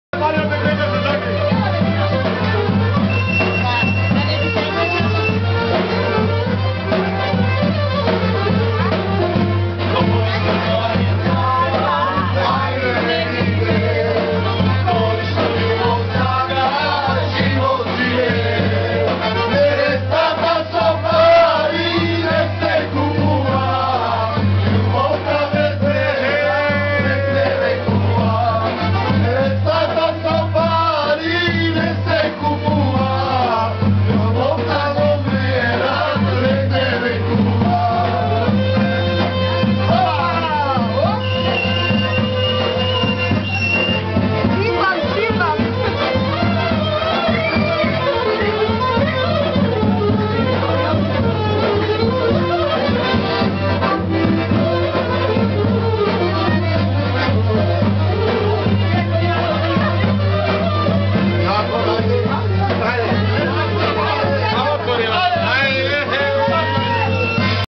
Makedonska Svadba - Bitola
Traditional custom of what goes on in Macedonia when the Groom goes to get the Bride from her place